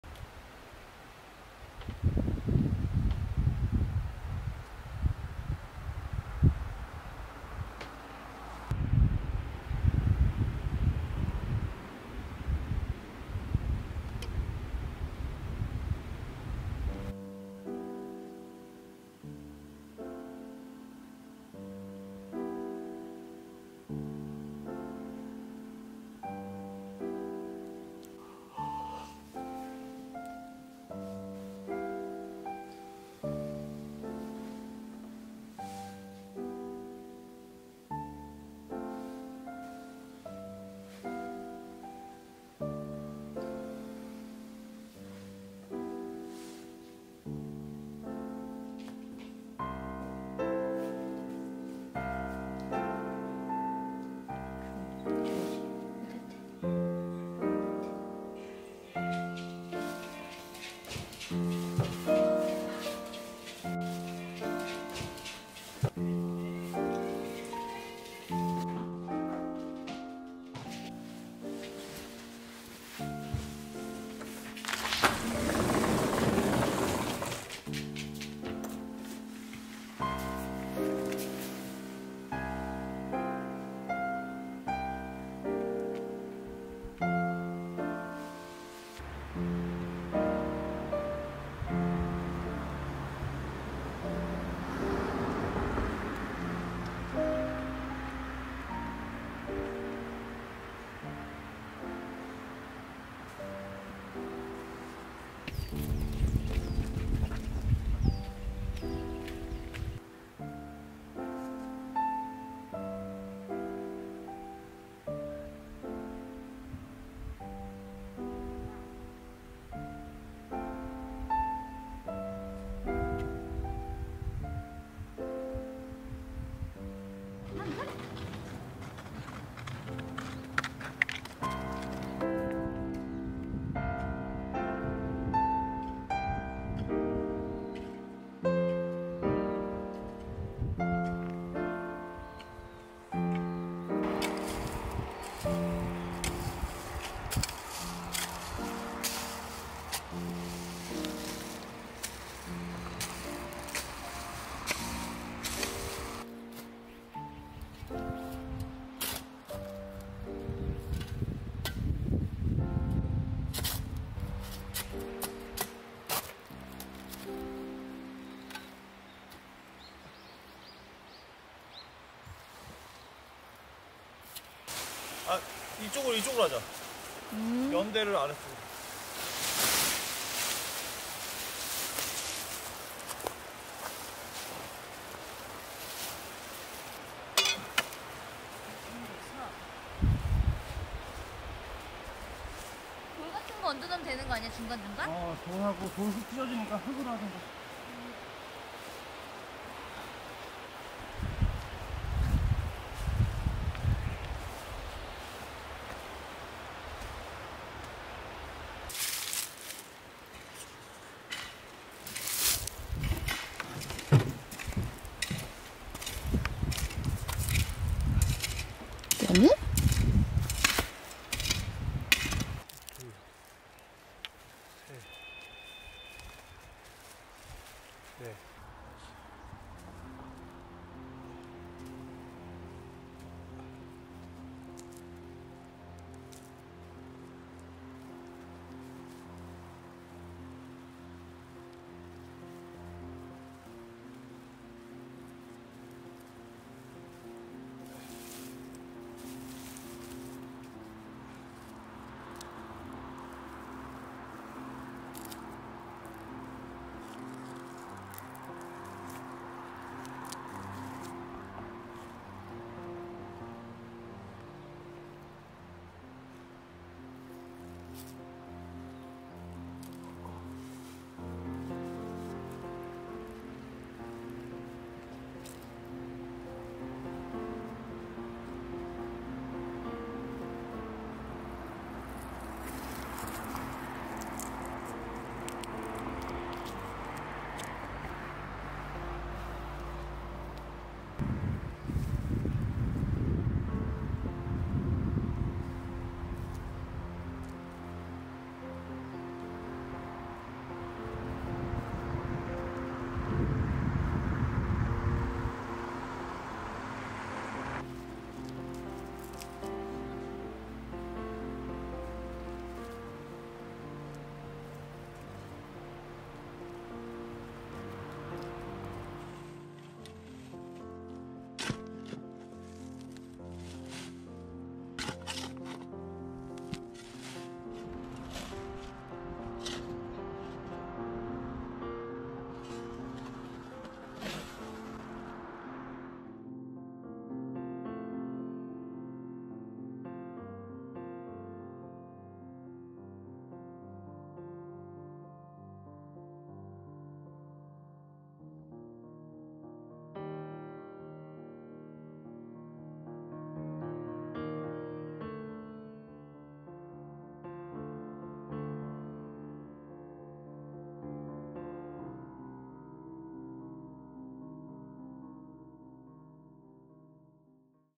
평온한 강아지들과 마늘심기 보더콜리와시골라이프
마늘이 잘 자라 주기를🙏

💜쨈과보리 인스타그램 💜